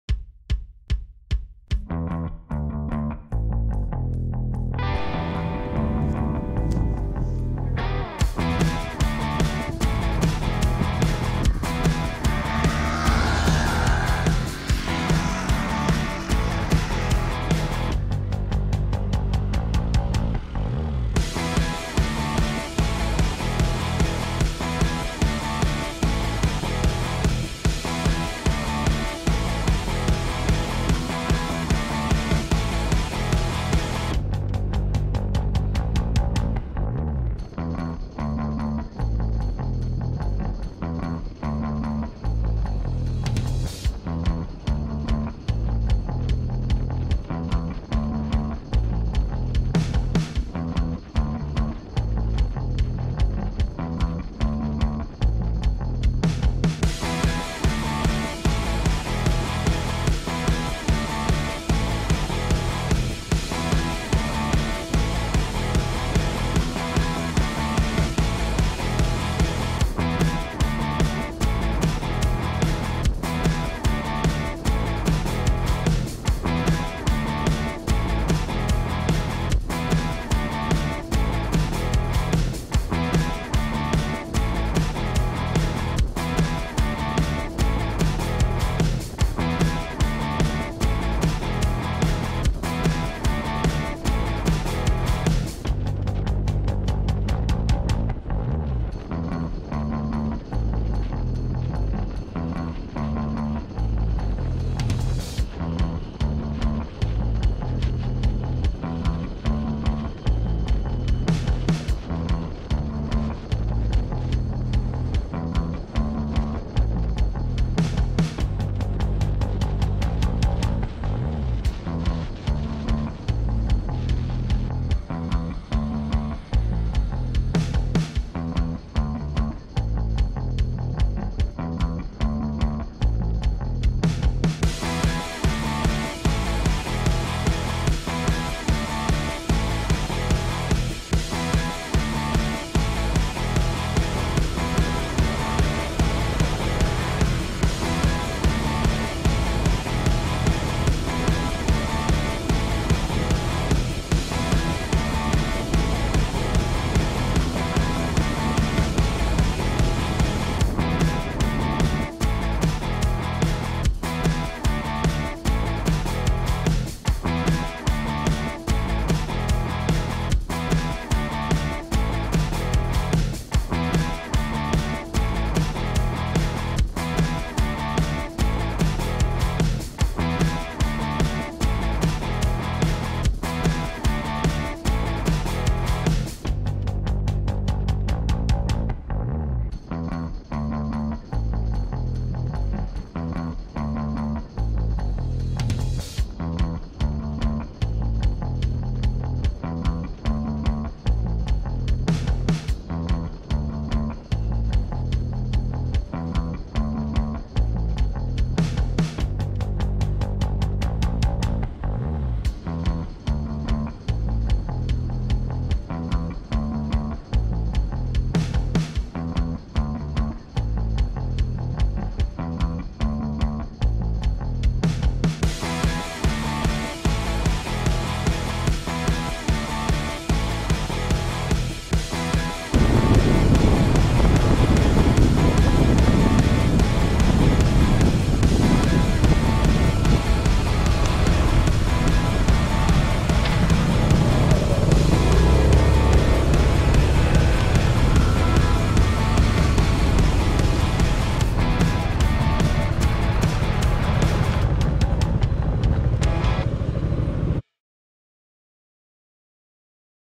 Rollertour zum Seehaus Pforzheim
Fahrt mit dem Scooter zum Seehaus Pforzheim
Vespa PX 80
Peugeot Django 150
Piaggio Beverly 350 (Kamera)